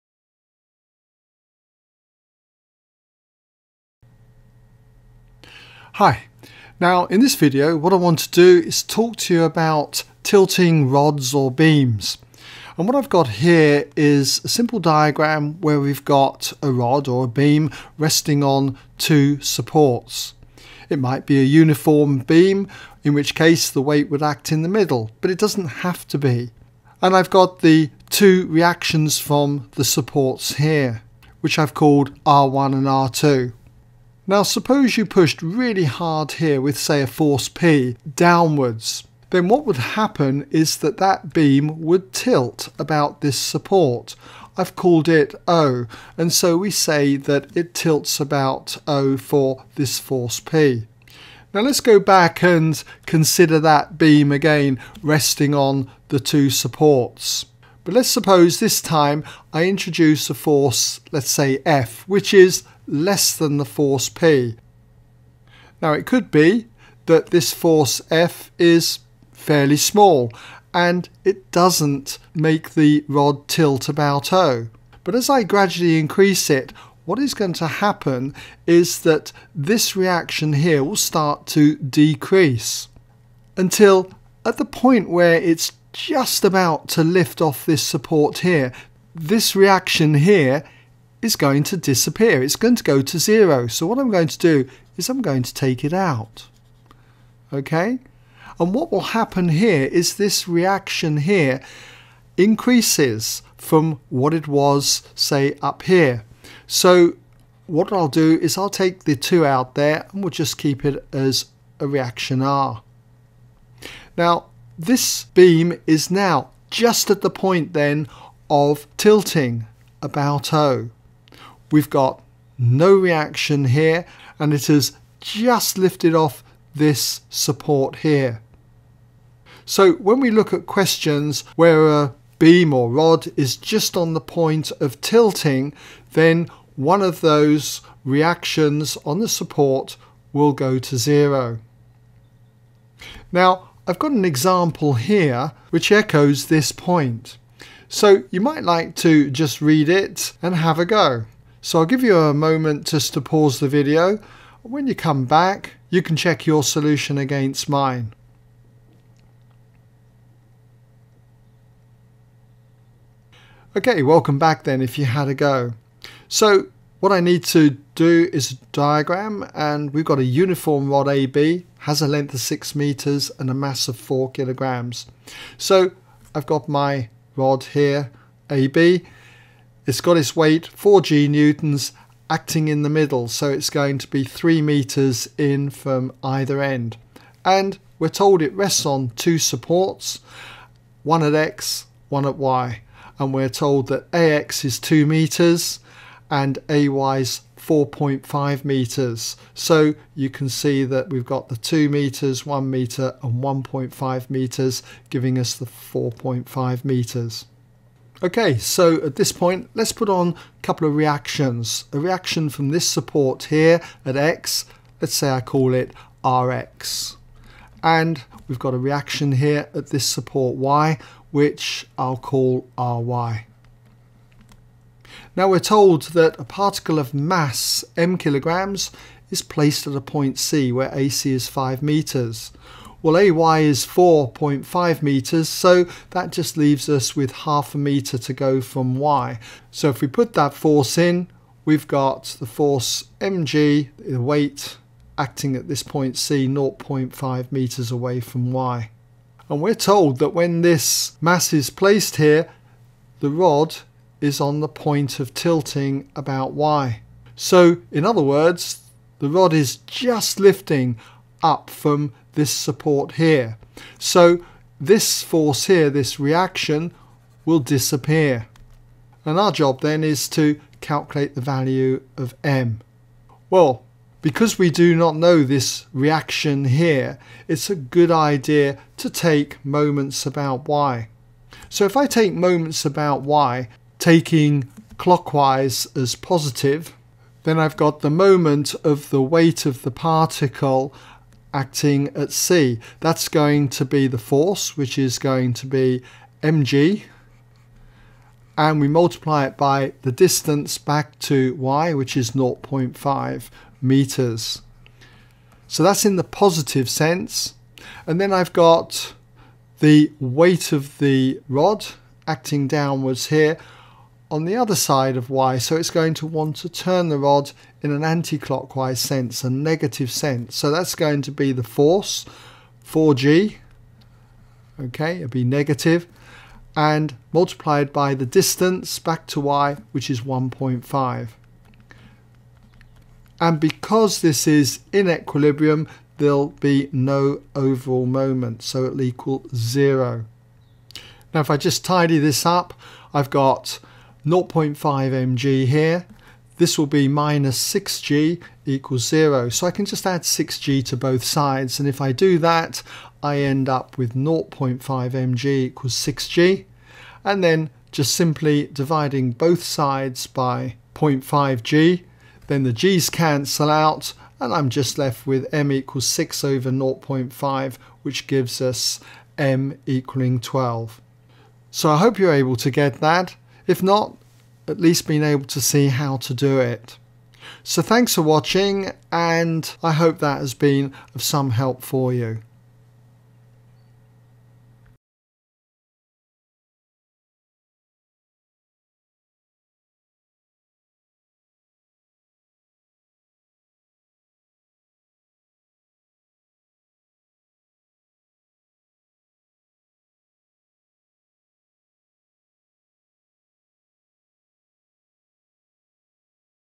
Moments of a Force | Tilting beams
Here I look at the moments of forces where tilting occurs about one support for a beam resting in equilibrium.